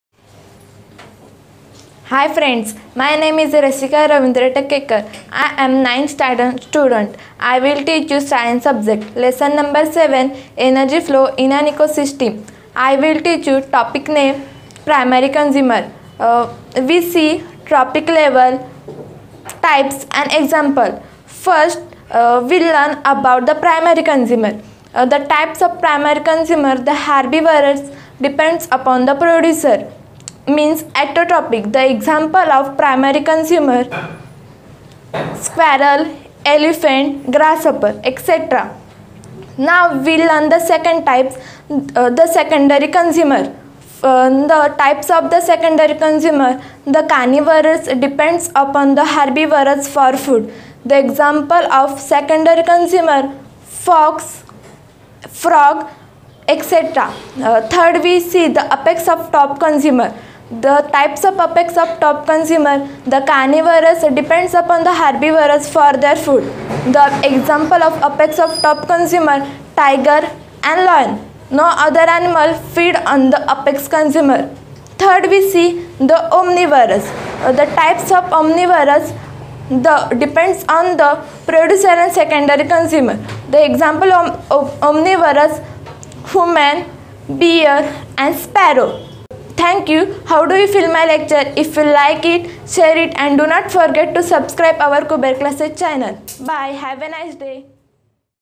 Primary & Secondary Consumer In Food Chain-Definition, Difference & Examples (Biology)
"Primary Consumer In Food"

The organisms that eat the producers are the "primary consumers" . ... The "primary consumers" are herbivores (vegetarians). The organisms that eat the "Primary Consumers" are meat eaters (carnivores) and are called the secondary consumers. The secondary consumers tend to be larger and fewer in number.
Primary consumers types are herbivores depend on the producer. "Examples Of Primary Consumers" are grasshopper, squirrel, elephant etc.
In secondary consumers types are carnivorous depend on herbivores for food. "Examples Of Secondary Consumers" frog, owl, fox etc.
In apex or top consumers types are carnivorous depends on herbivores for their food. Examples of apex or Top Consumer tiger and lion etc. No other animal feed on apex or top consumers.
In omnivores types depend upon producer and secondary consumers. Examples of omnivores human, Bear, sparrow etc.
Sample answers:
"Primary Consumers Examples"  cows, rabbits, tadpoles, ants, zooplankton, mice.
"Secondary Consumers Examples"  frogs, small fish, krill, spiders. Tertiary consumers: snakes, raccoons, foxes, fish. Quaternary consumers: wolves, sharks, coyotes, hawks, bobcats.
"Secondary Consumers" are an important part of the food chain. They control the population of "Primary Consumers" by eating them for energy. Secondary consumers also provide energy to the tertiary "Consumers" that hunt them. … By the time a secondary organism eats, they only receive 1% of the original energy available. (biology)
(आपला आभारी आहे, धन्यवाद 🙏)
TIMESTAMPS 👇

0:00 Primary Consumer Introduction-
0:15 Primary Consumer-
0:33 Primary Consumer Examples-
0:40 Secondary Consumer-
0:50 Secondary Consumer examples-
0:58 Apex Consumer-
1:07  Apex Consumer Examples-
1:15 Omnivores Consumer-
1:23 Omnivores Consumer Examples-